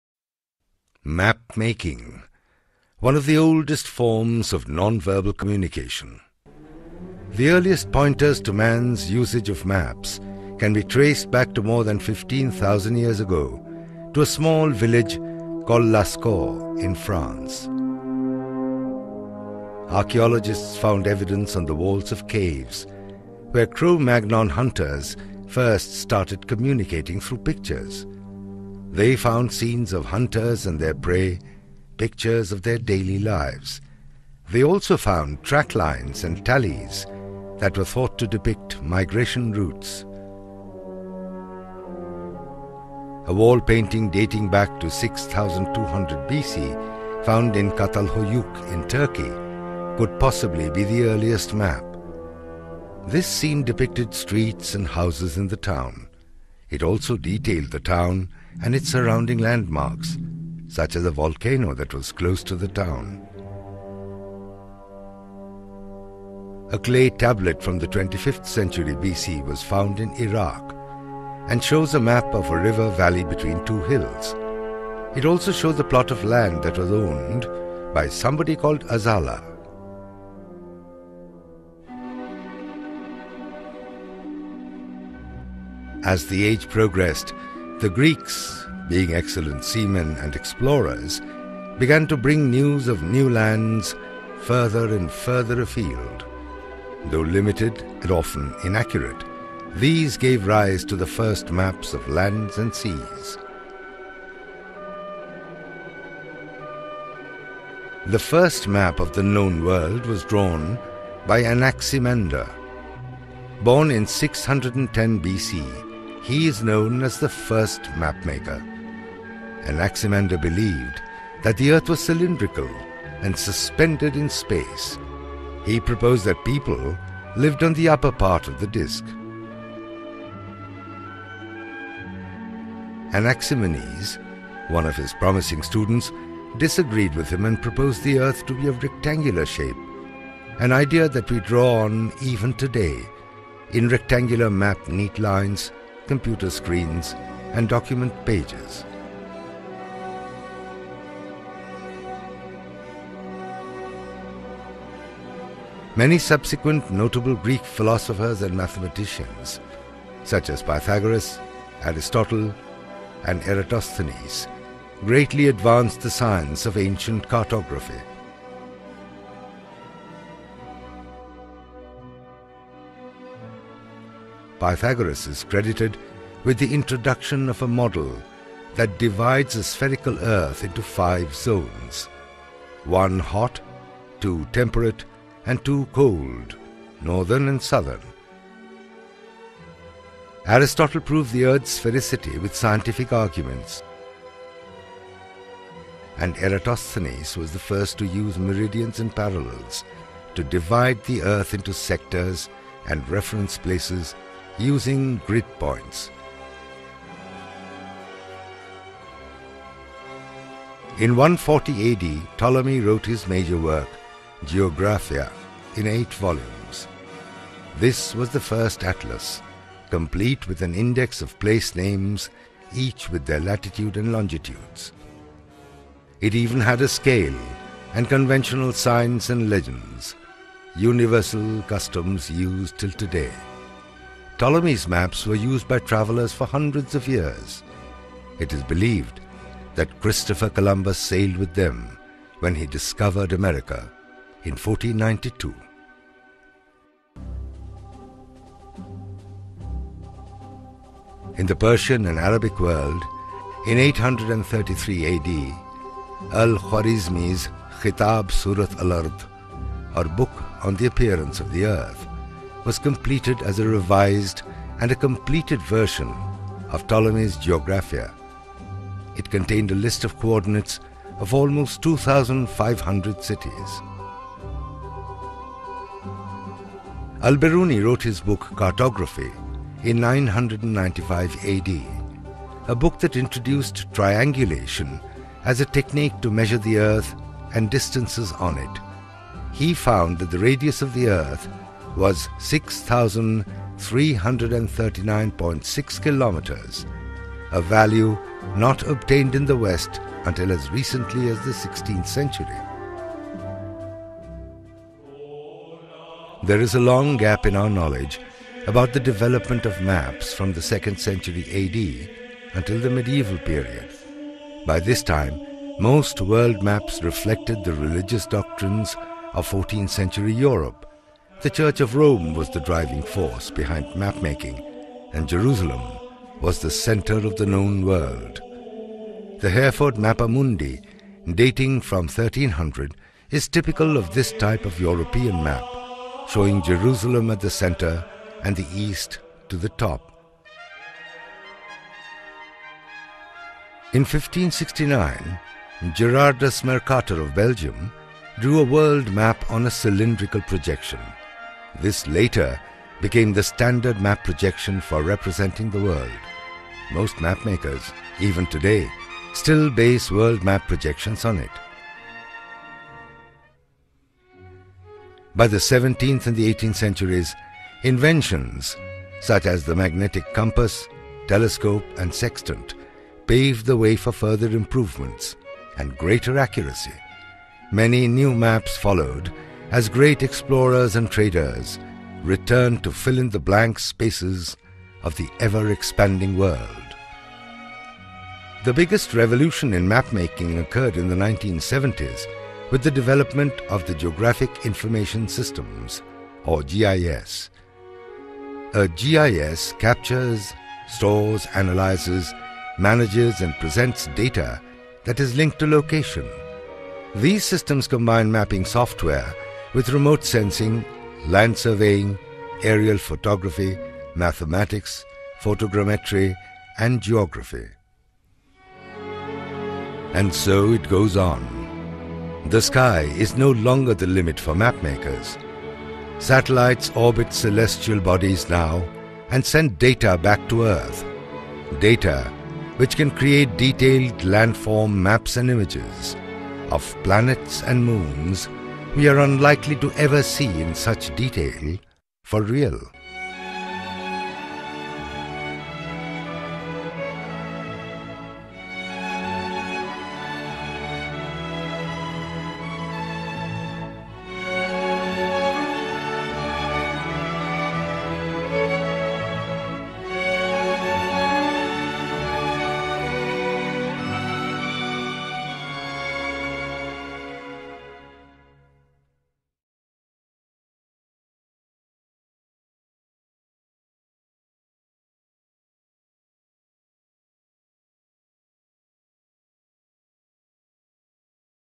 Η ιστορία της χαρτογραφίας - Ελληνικοί Υπότιτλοι
Μια συνοπτική περιγραφή της ιστορίας της χαρτογραφίας, από το Σπήλαιο του Λασκώ μέχρι και τα σημερινά Συστήματα Γεωγραφικών Πληροφοριών. Οι χάρτες κάθε εποχής δεν αποτελούν μόνο αναπαραστάσεις τόπων και γεωγραφικών σημείων - αντικατοπτρίζουν επίσης τη φιλοσοφία του κάθε πολιτισμού, καθώς και τις αντιλήψεις αλλά και τις προσδοκίες της εκάστοτε κοινωνίας.